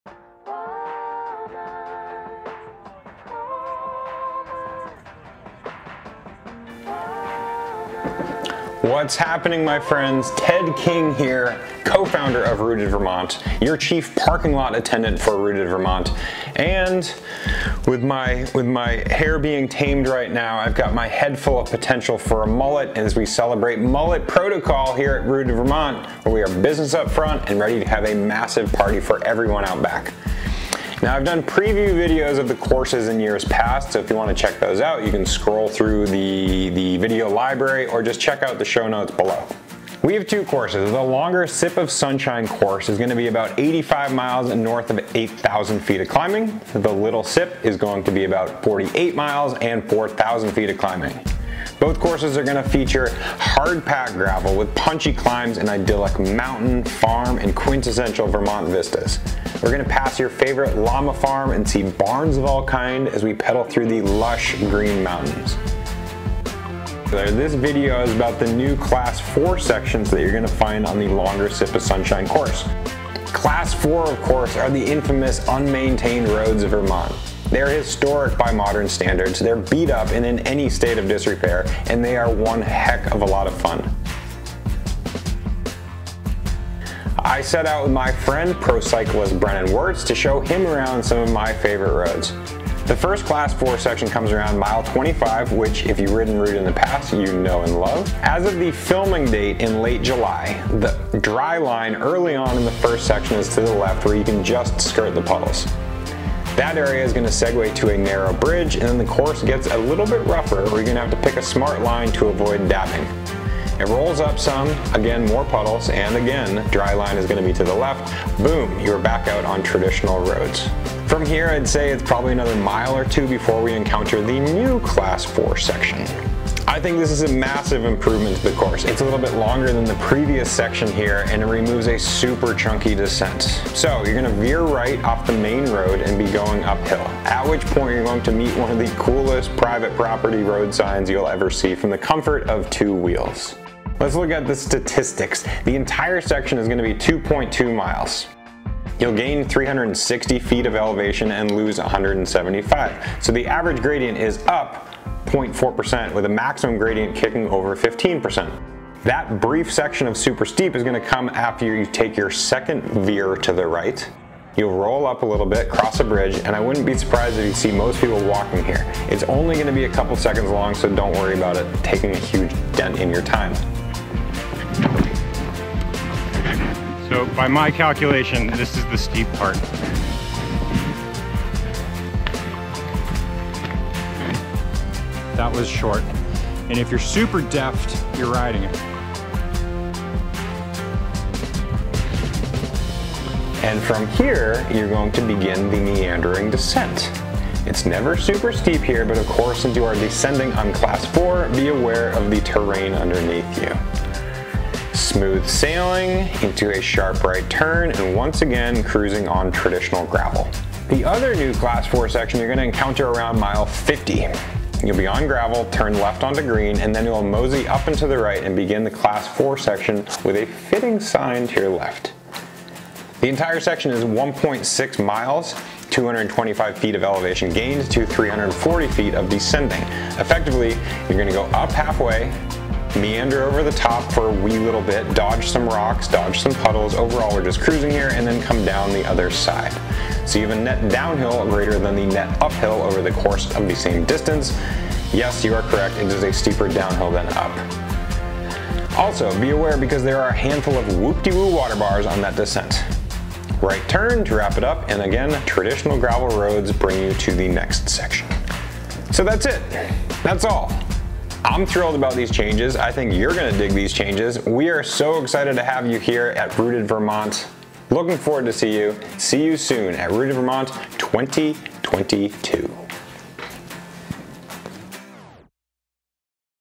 Course Preview Rooted Vermont 2022 to see the New Class 4 Sections
Rooted Vermont 2022 is right around the corner and this video will help you see what's in store for the slight tweaks into the coming year.